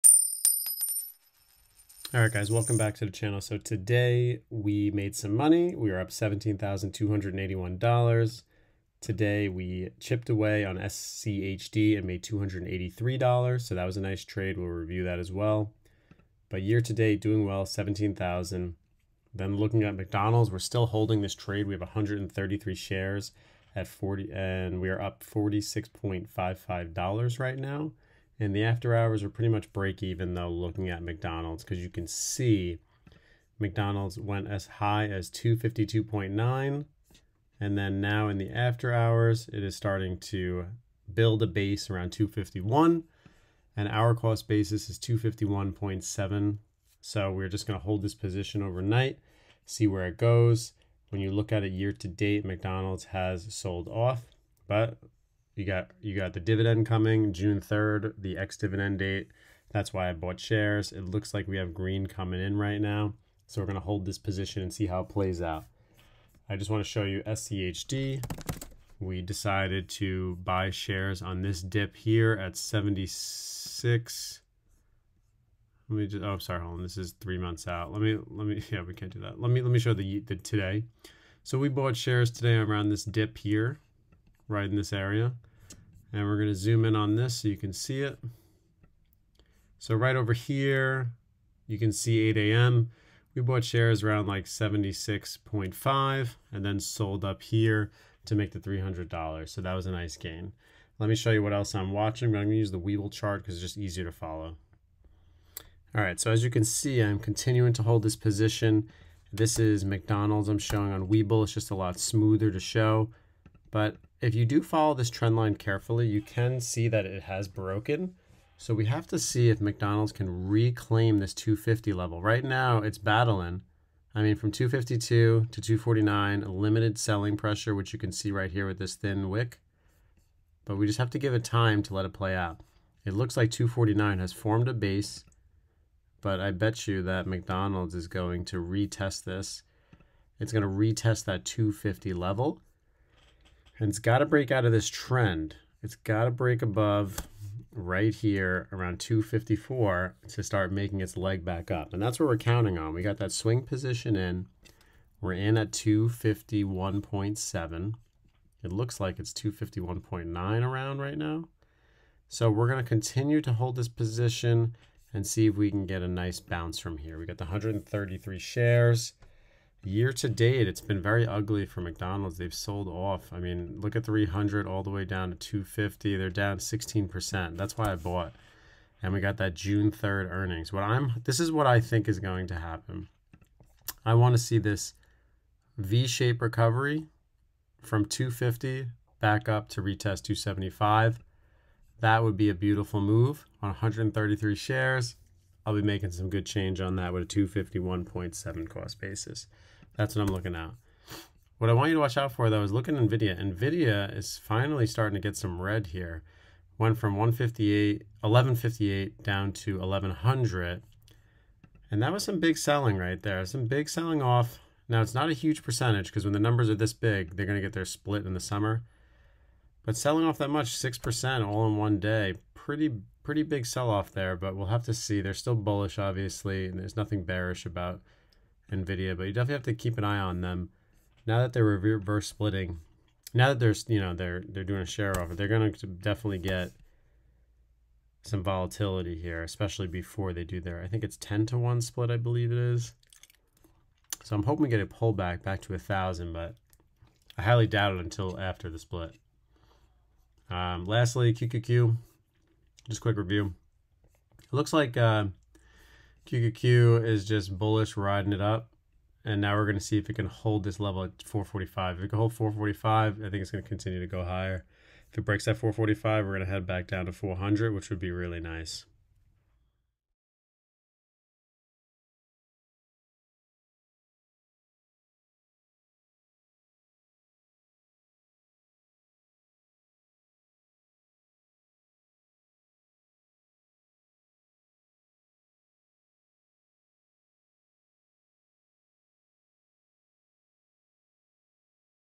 P/L 2024 - Still holding McDonalds
In this video, I review my SCHD trade and my position on MCD stock.